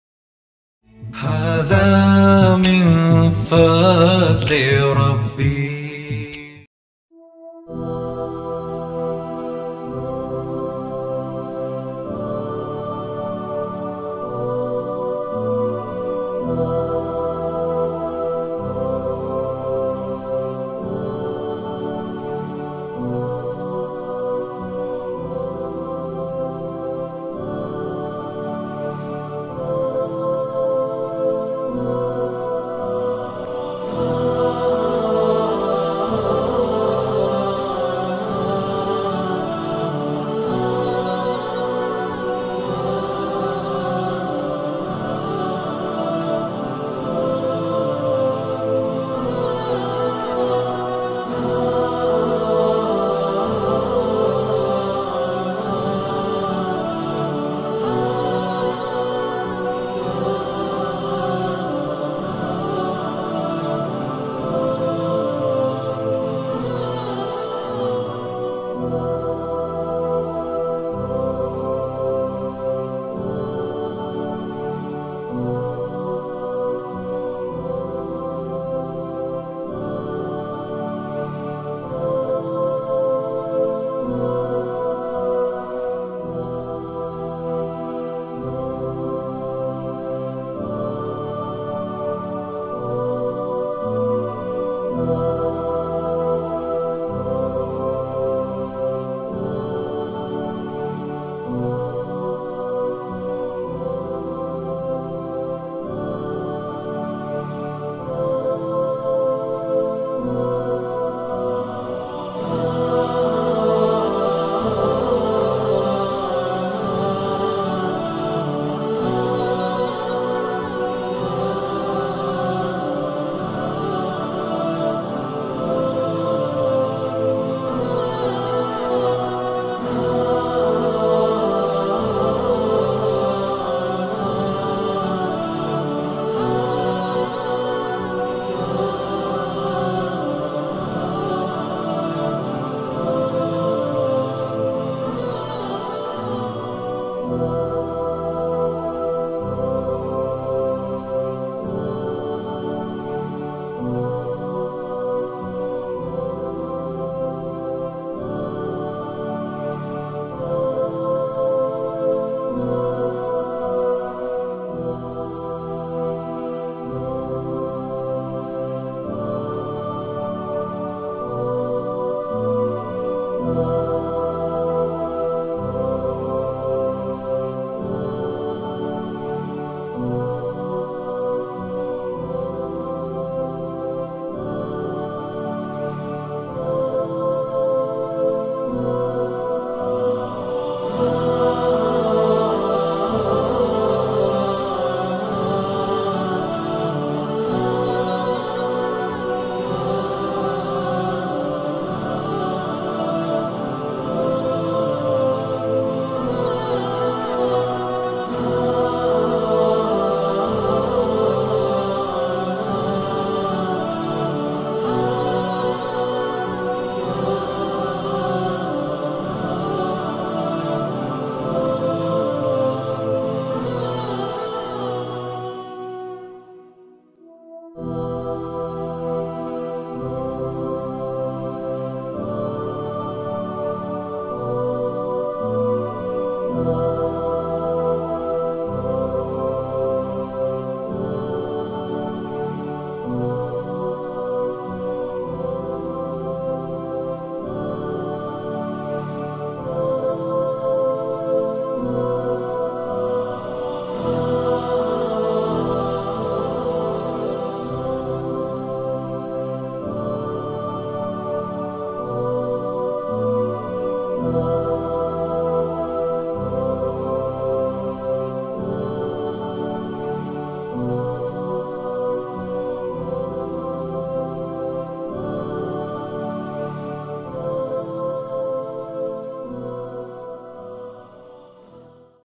Haza Min Fazli Rabbi - هذا من فضل ربّي Islamic Calligraphy | ART MAJOR
This Painting was made For a Customer on Order ..
This Painting was completed In 2 days With Acrylic Colours On Canvas

For Purchasing Purposes Contact On :
Sorry !! This Painting was Made For a Customer on Order And has been SOLD.